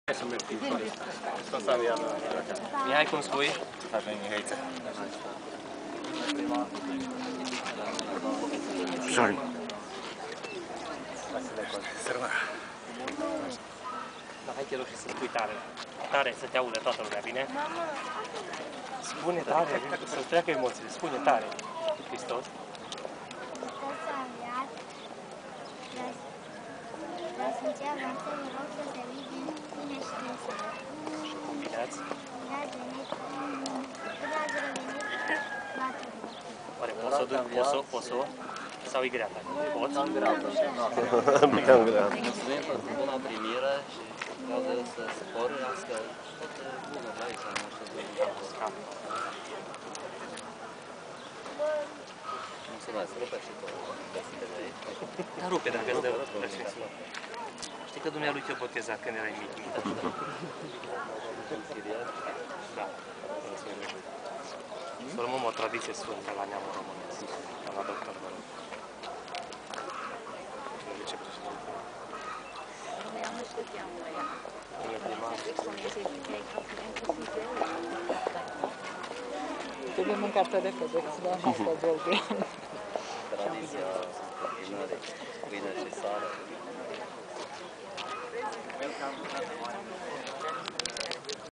Inaugurarea Centrului "Sf. Irina" Turda
Centrul de Recuperare Psiho-Neuro-Motorie pentru Copii cu Dizabilităţi „Sfânta Irina, de pe str. Bariţiu nr. 39 A, Turda a fost inaugurat miercuri, 22 aprilie.